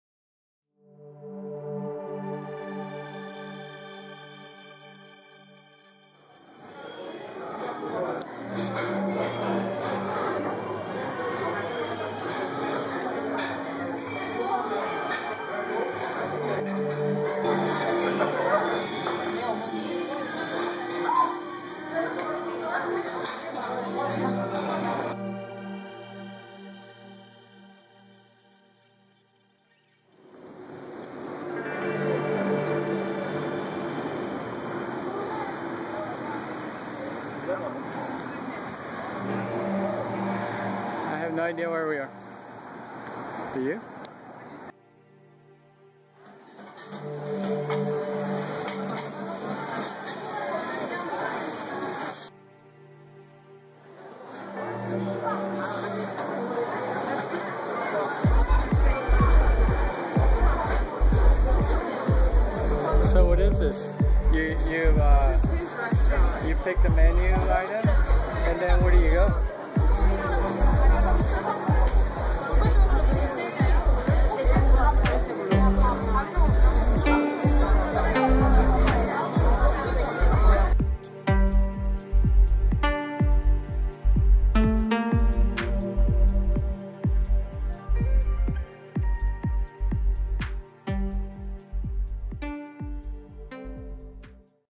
TurboVids - Travel - Seoul conveyor belt sushi restaurant
While traveling through Korea, found some conveyor belt sushi or sushi train restaurants (AKA Kaitenzushi).

They are interesting as you simply sit next to the sushi as it goes by, and you pick the sushi you want. You stack the plates right next to you, and they charge you based on the plates.